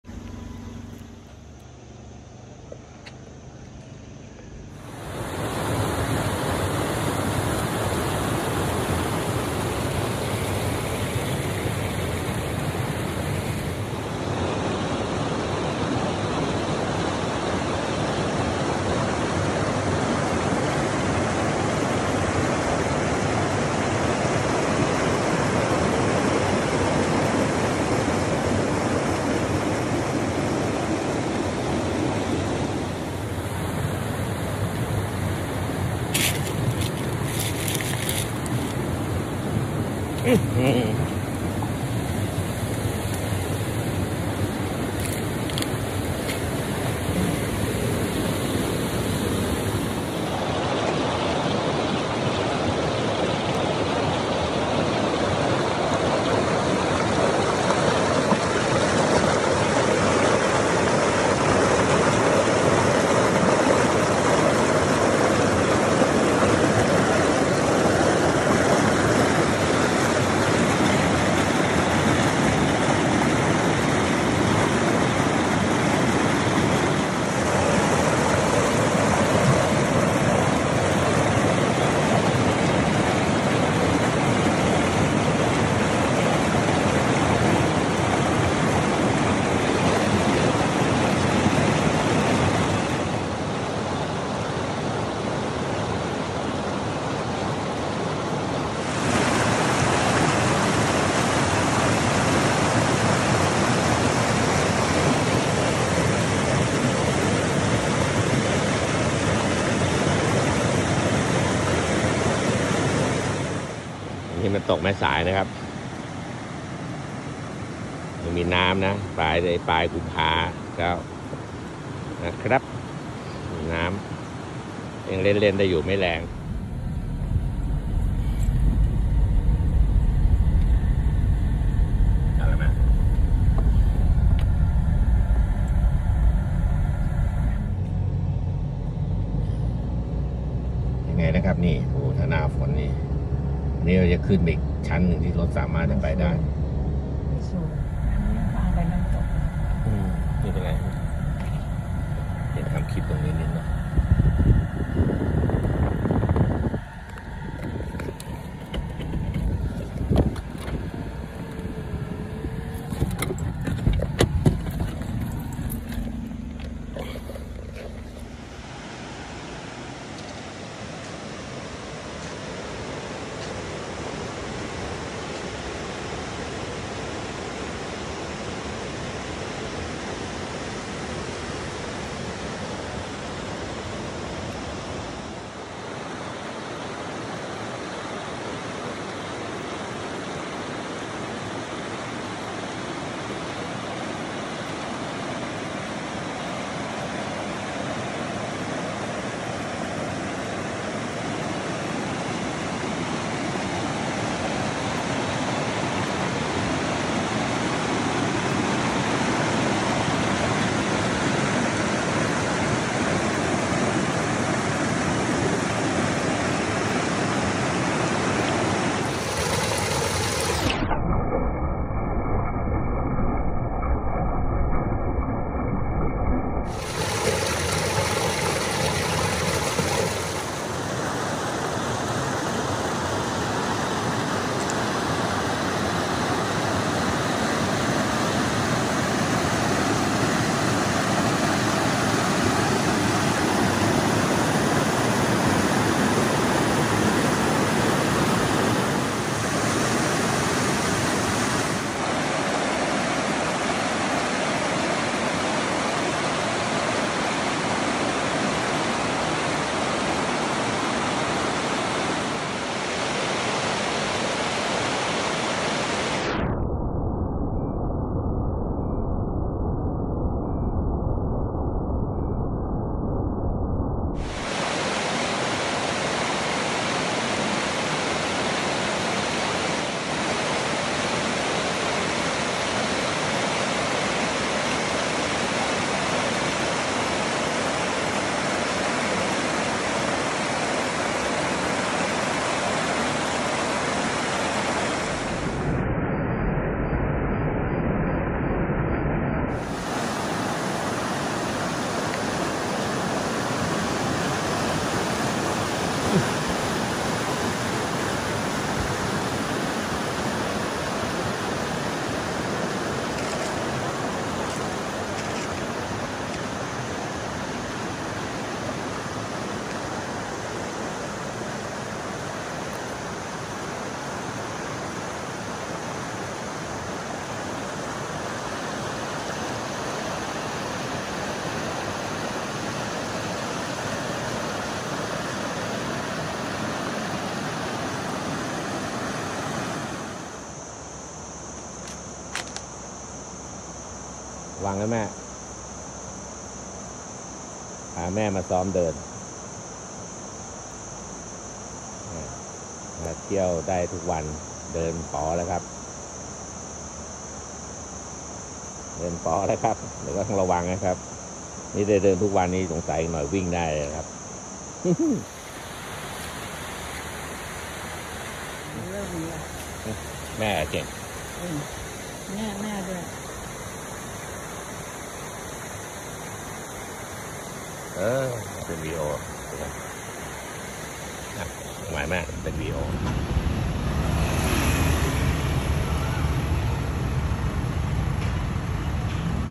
พาแม่ไปเที่ยวน้ำตกแม่สาย ต้องดู Tongdoo 必看视频
ถึงเวลาที่ต้องพาแม่ไปเที่ยว แม่บอกตอนเราเด็ก พาไปเที่ยวยากกว่านะ